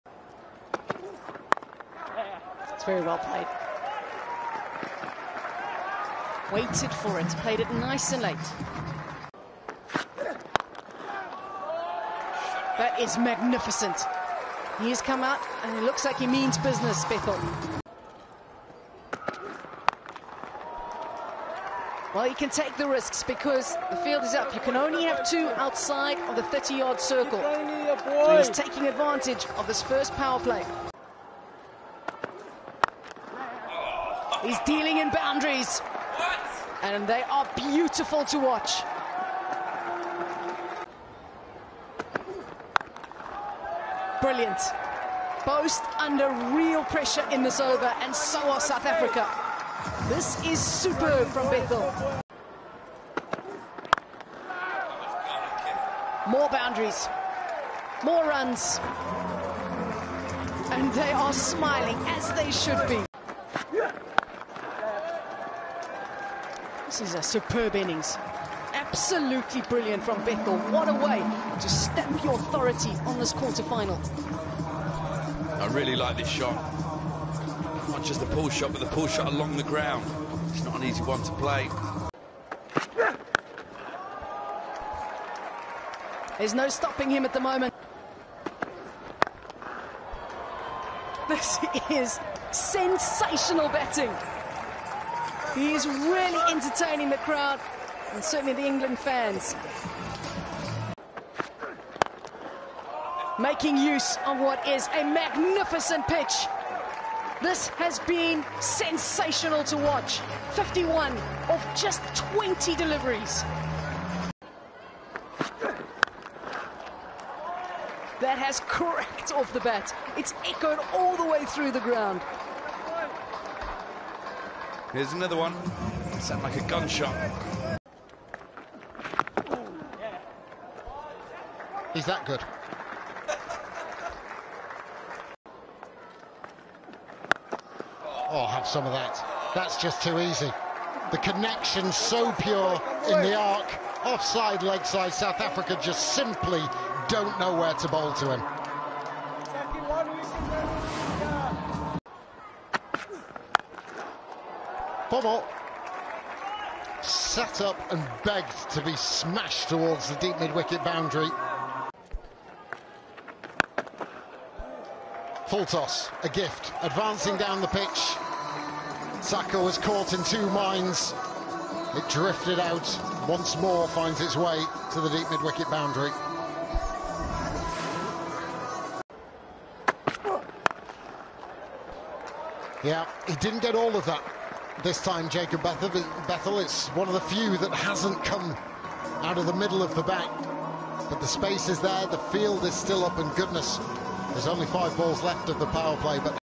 Jacob Bethell's quick-fire 88 v South Africa | U19WC 2022
Blazing 88 from just 42 balls!

Before he became England's Test No.3, Jacob Bethell shone at the U19 Men's Cricket World Cup back in 2022!